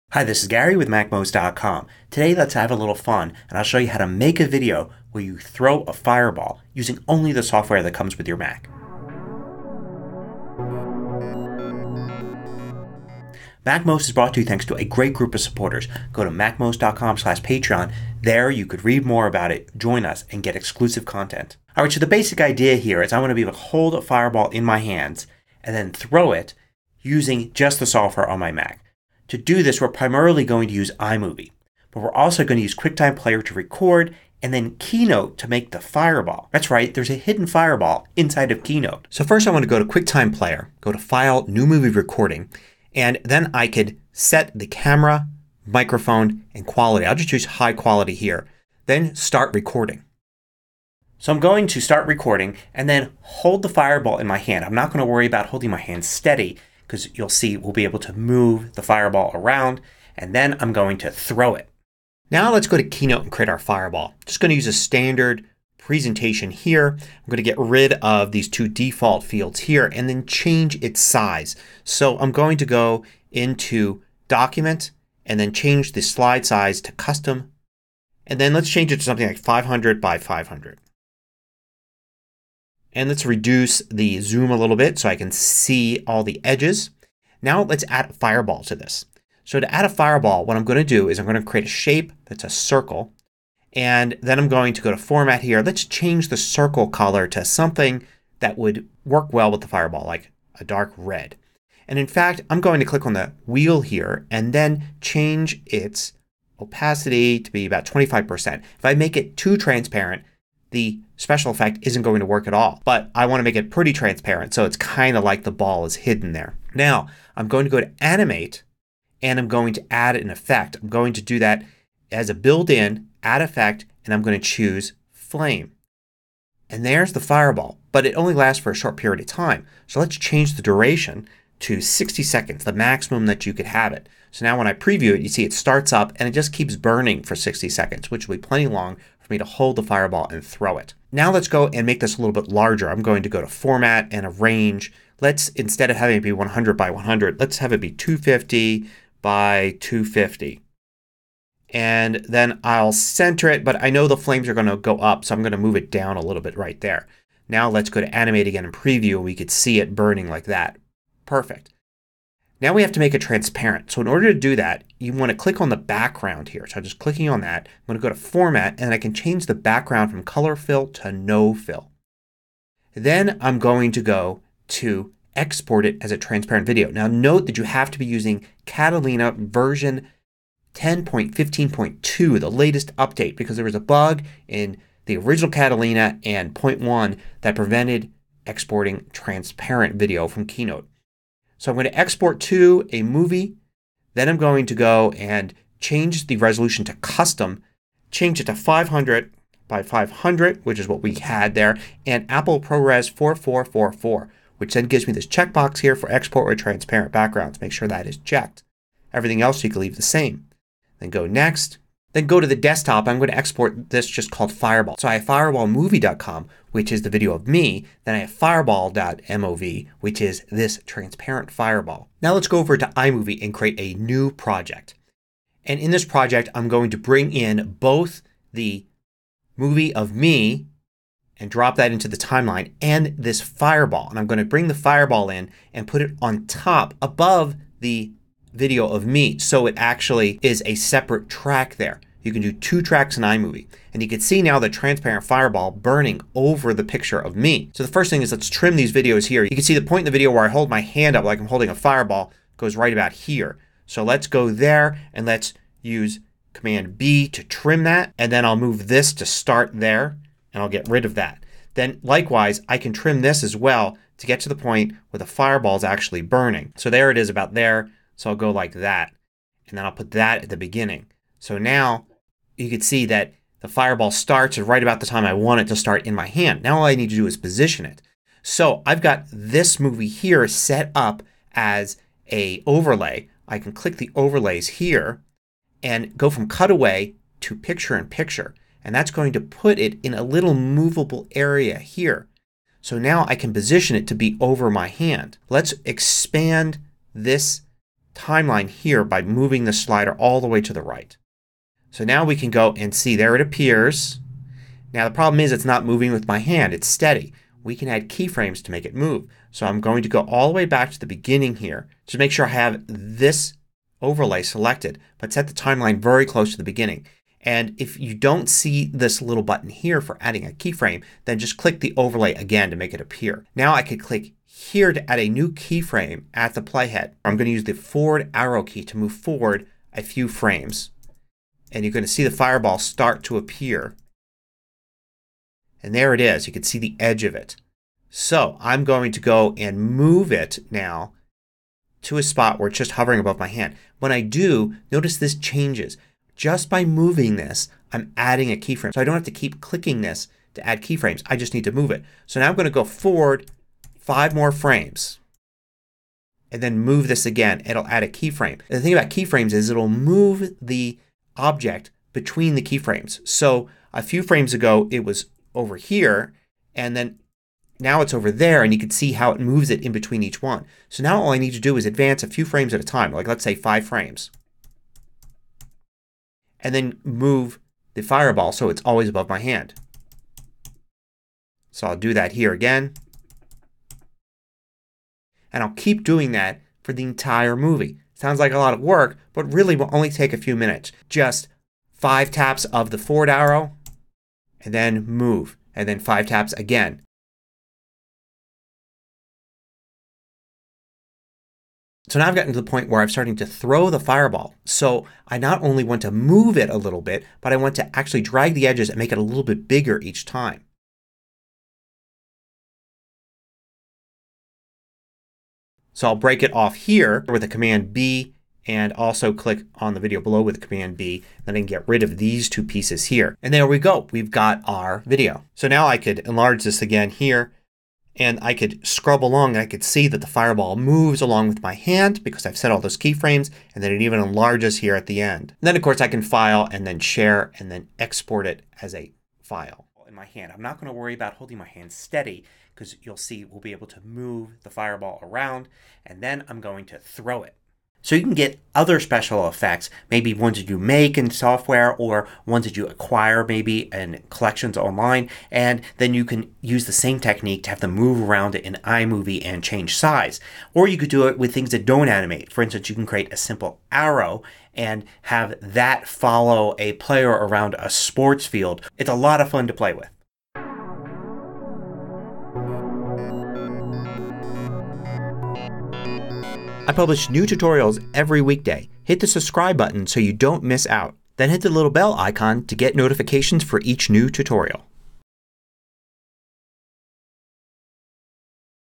Create a Magic Fireball Video Using the Software On Your Mac
You can create a fireball in Keynote and then export it as a transparent video. You can then record yourself with QuickTime Player and place the fireball in your hand. It can move with you and you can even throw it if you put the time in to create animation keyframes in iMovie.